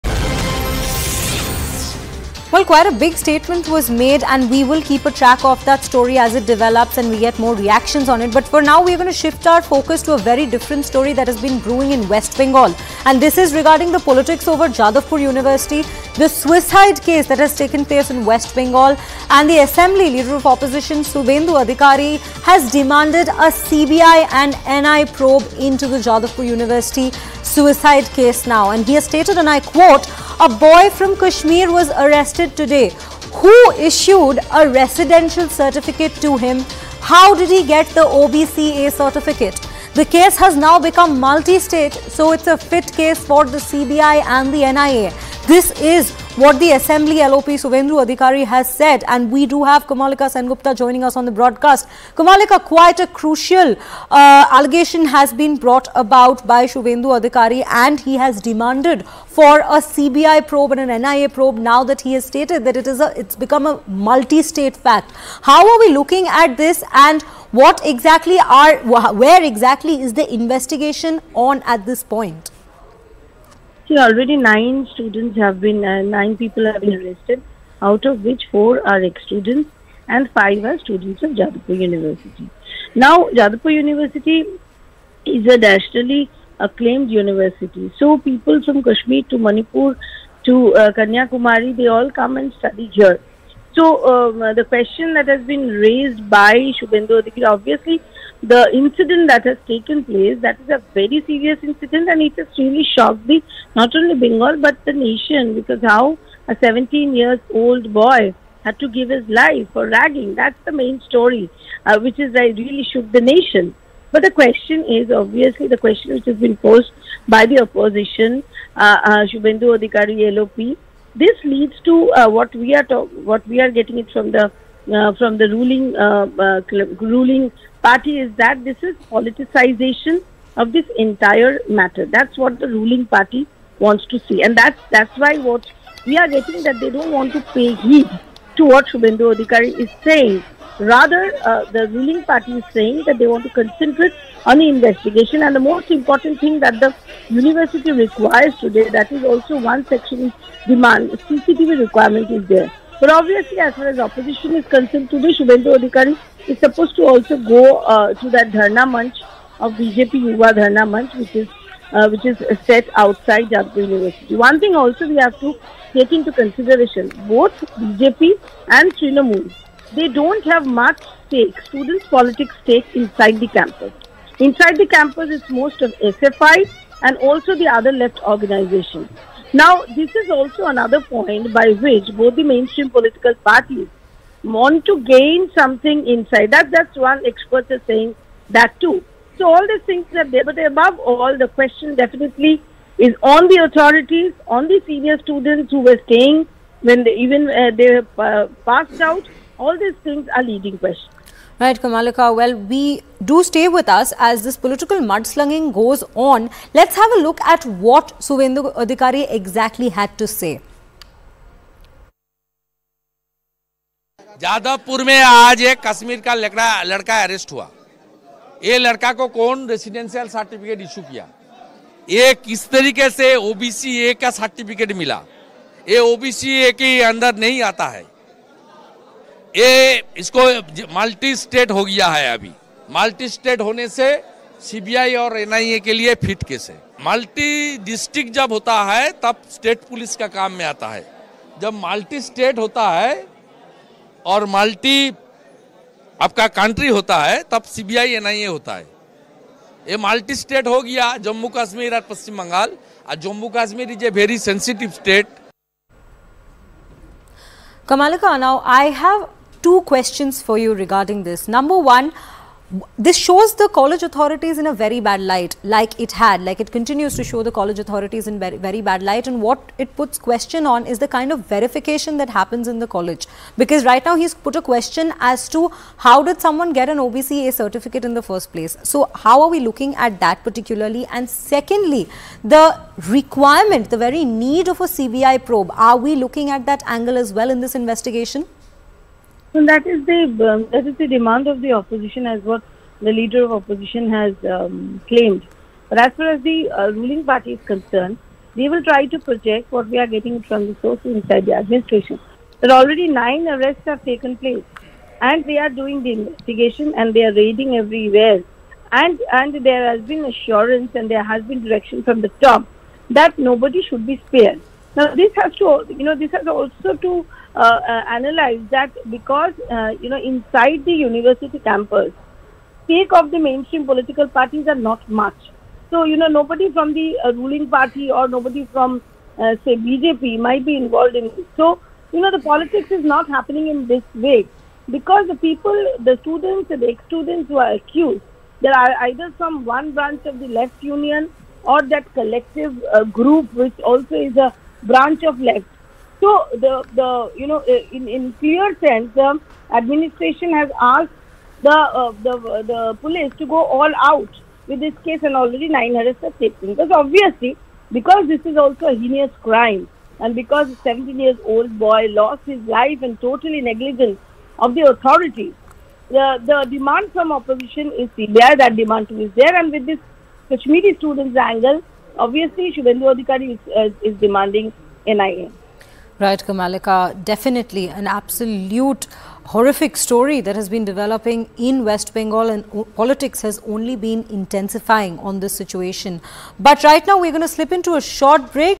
West Bengal News | Kolkata News | BJP Demands CBI Probe In Jadhavpur University Case | News18
Student unrest contours in Jadavpur University on the death of 17 years old student. TMCP is sitting on dharna outside University, BJP Yuva morcha to sitting in dharna outside University. Suvendu Adhikary might visit BJP Yuva protest. Inside University FAS, AISA, SFI all in protest. Justice for 17-year-old is students demand. 9 people are arrested out of which 4 are external 5 are present day students. Police has spoken to Registrar and one professor. Dean of students did not visit on Wednesday sighting student unrest.

n18oc_india